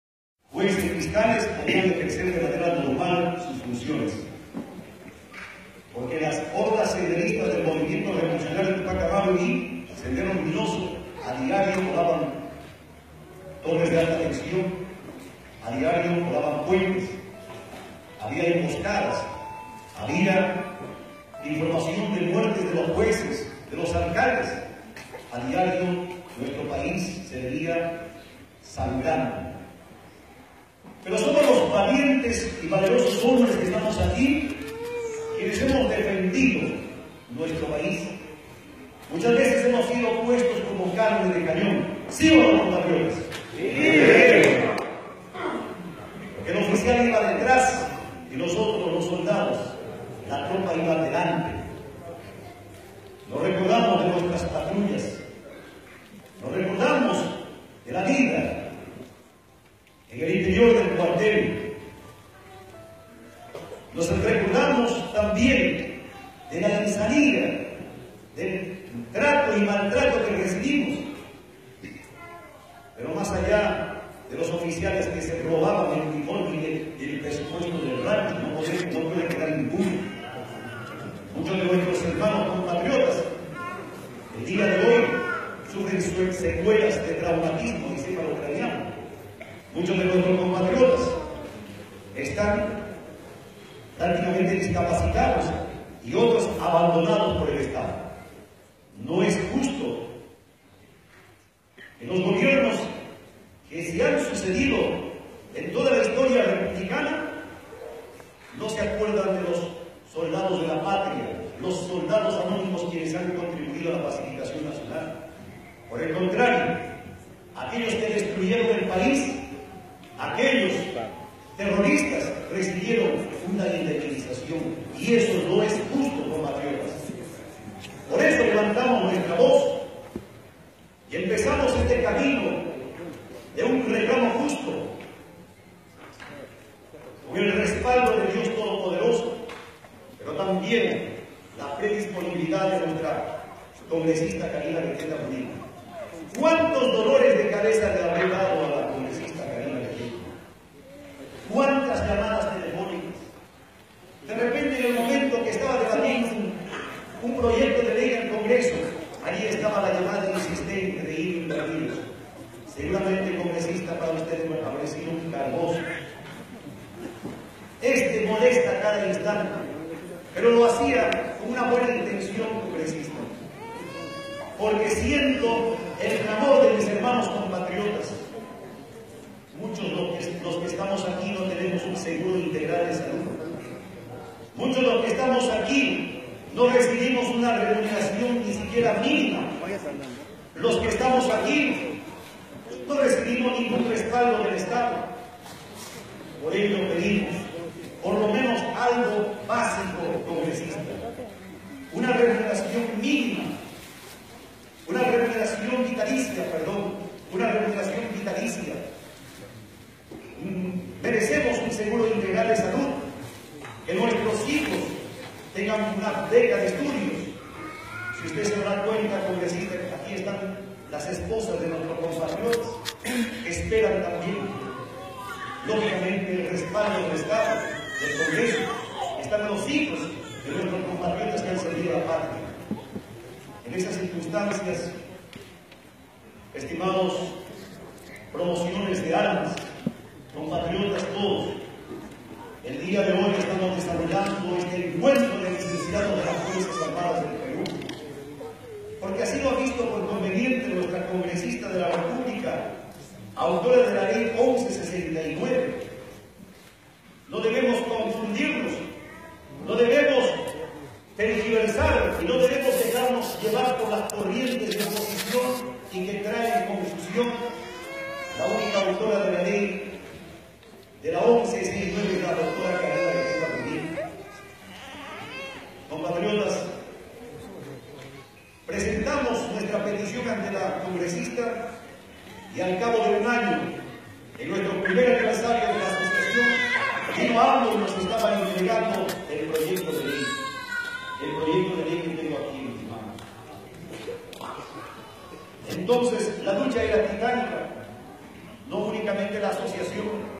DOCTORA KARINA BETETA SE REUNIÓ HOY DÍA CON LICENCIADOS DEL EJÉRCITO
LA CONFERENCIA DE LOS LICENCIADOS EN HUÁNUCO  DURÓ POR BUEN TIEMPO , EXPLICA  CONGRESISTA KARINA BETETA SOBRE EL PROYECTO DÉ LEY 1169    BENEFICIO PARA LOS LICENCIADOS DEL EJÉRCITO QUE LUCHÓ CONTRA EL TERRORISMO , SUSCRÍBETE PARA QUE SIGUES VIENDO MÁS VIDEOS NUEVOS CALIENTITOS SOBRE EL PROYECTO DE LEY